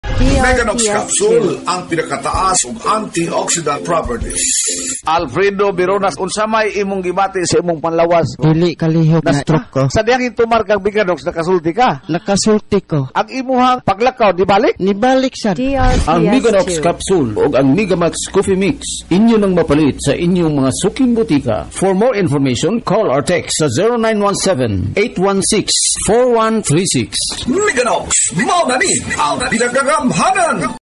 Meganox Testimonial Radio Ad 2010s-2024 31s (in Cebuano Version 6)
Ito po ang 2010s-2024 Testimonial Radio Ad ng Meganox (Version 6).

DRPS Two 6933: Meganox Testimonial Radio Ad 2010s-2024 31s (in Cebuano Version 6)